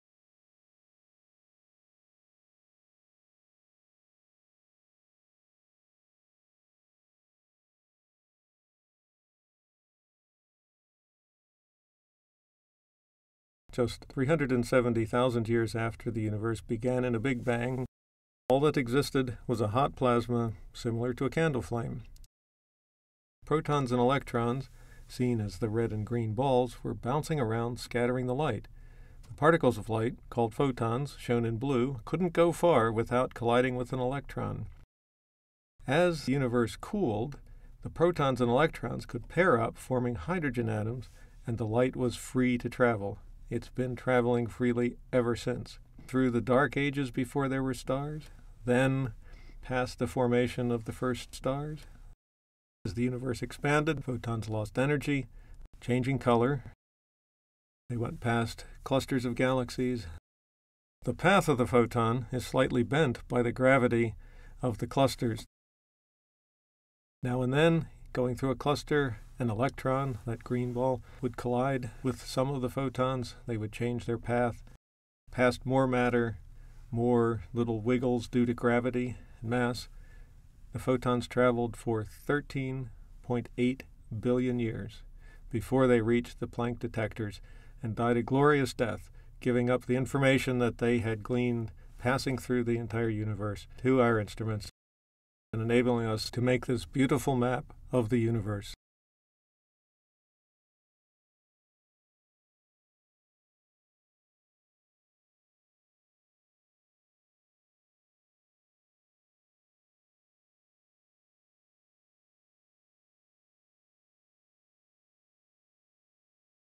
A Journey of Light Through Time and Space
Follow the "life" of a photon, or particle of light, as it travels across space and time, from the very early universe to the Planck space telescope.